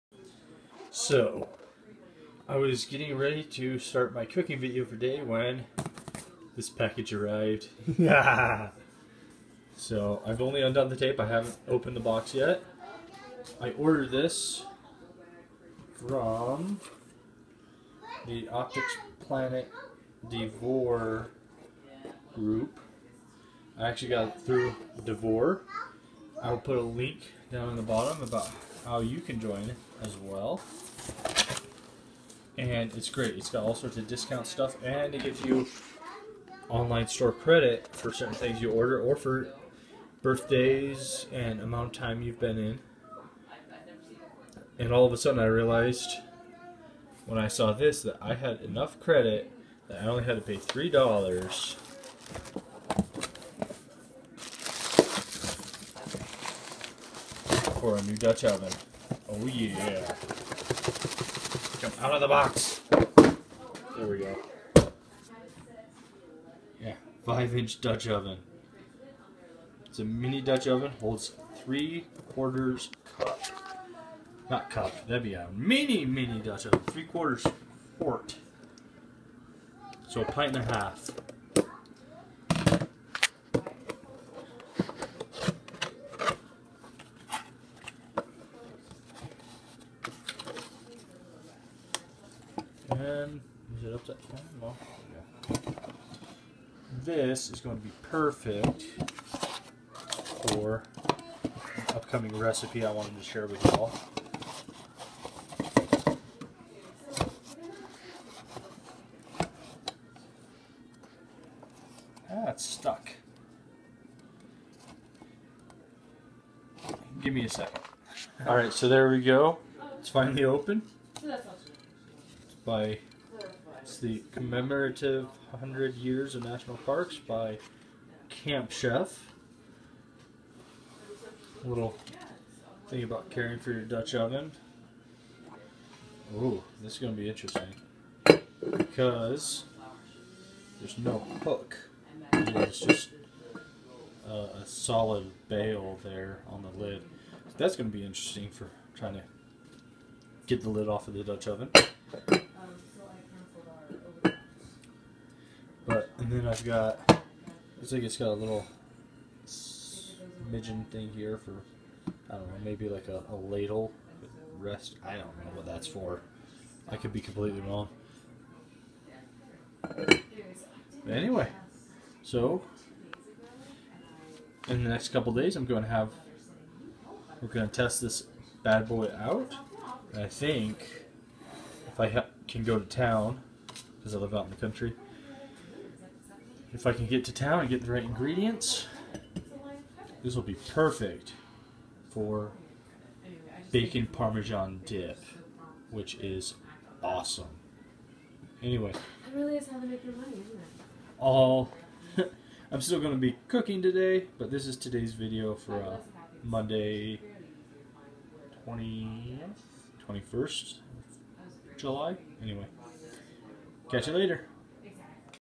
Unboxing a New Oven
Mat gets a new Dutch oven in the mail.